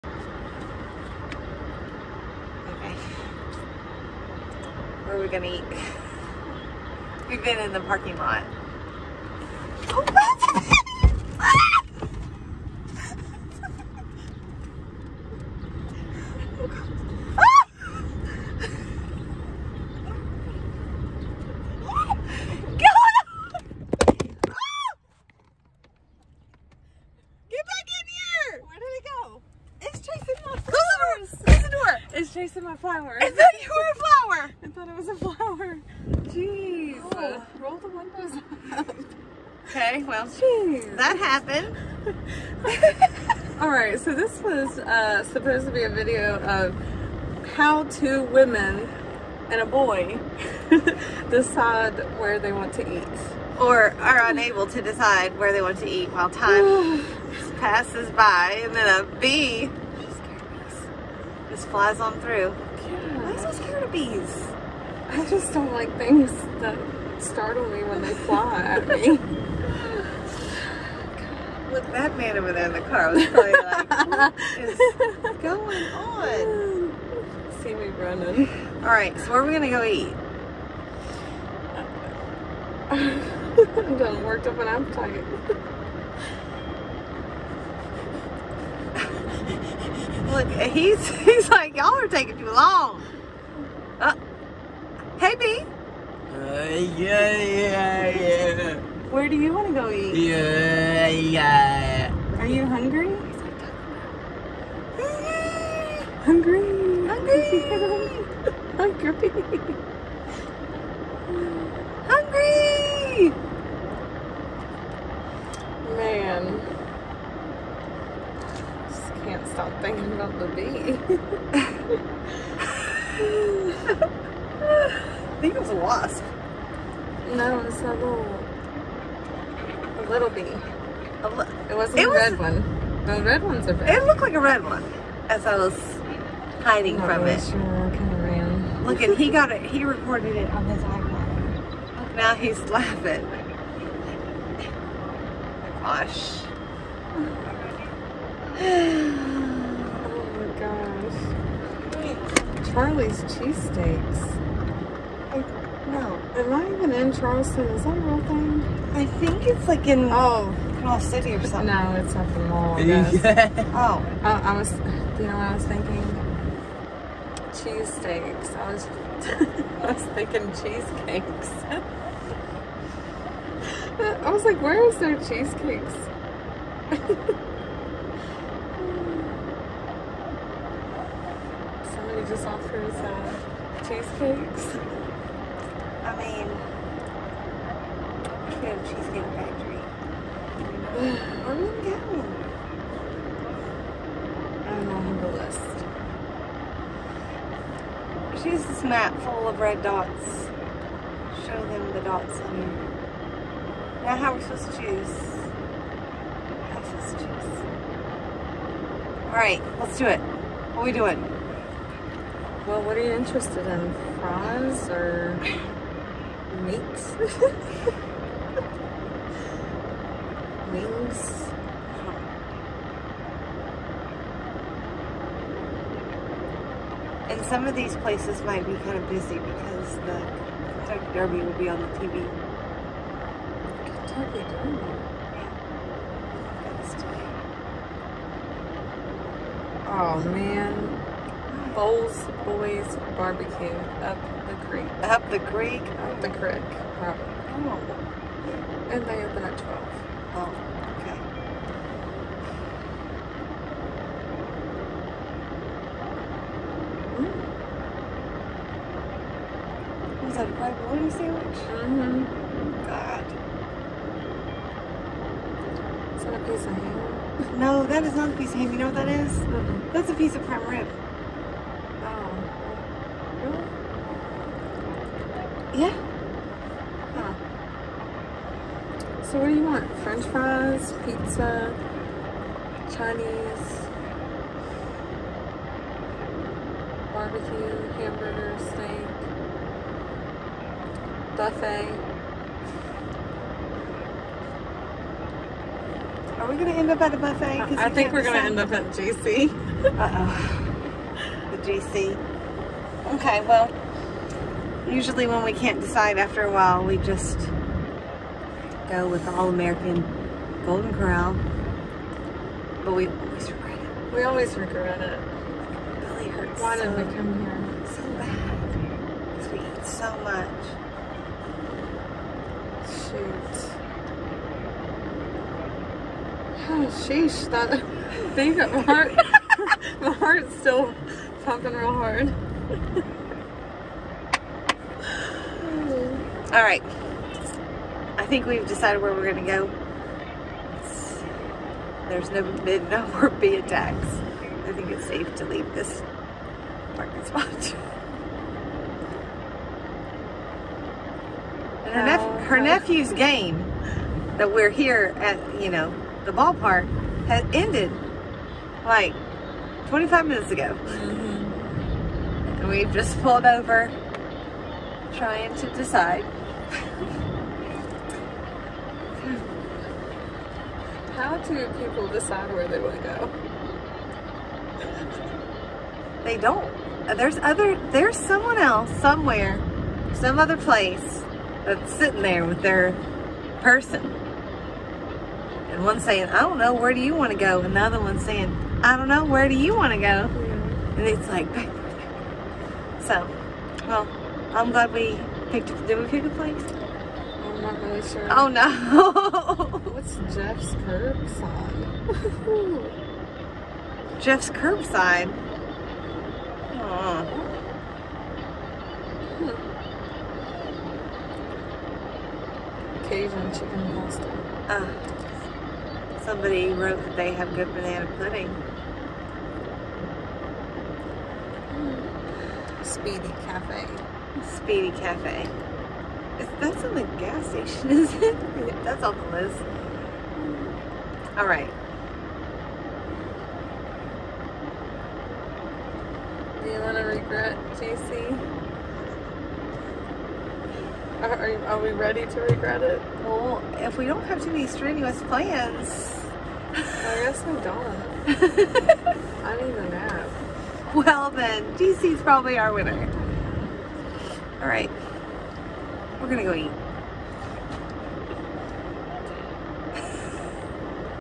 Bee interrupts lunch decisions.
A wondrous bee rudely interrupts a family deciding where to eat lunch. Driver bolts, lol! Once returning to the car, this is what it's like when 2 women choose where to have lunch. Do all couples have trouble choosing a place to eat?

Where will they choose? A restaurant, a burger joint, a speedy cafe, or a hole- in-the-wall diner? Will they choose steaks with fries, a cheesy pizza, or a buffet with many food options?

Typically our favorite place to eat would include tasty, buttery rolls. However, Bri does try to avoid those because she has a wheat intolerance. One of her favorite dishes would consist of a medium-well-seasoned steak with seasoned fries and a salad on the side, with a unique sauce to dip in. One of Tab's favorite meals would include pot roast with potatoes, buttery rolls, and a chocolatey dessert. B, Tab's son loves a couple of warm tacos from Taco Bell.

What do you think? What's your favorite go-to comfy food?